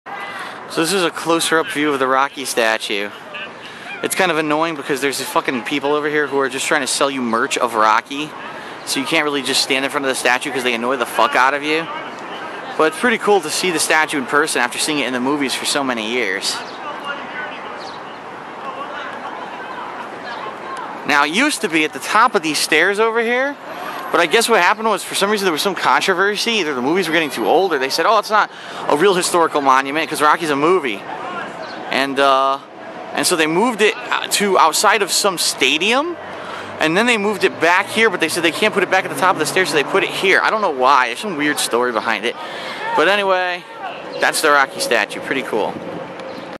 Philadelphia Trip 2013 Day 2 - Rocky Statue
This is vlog footage of my trip to Philadelphia, PA during May of 2013.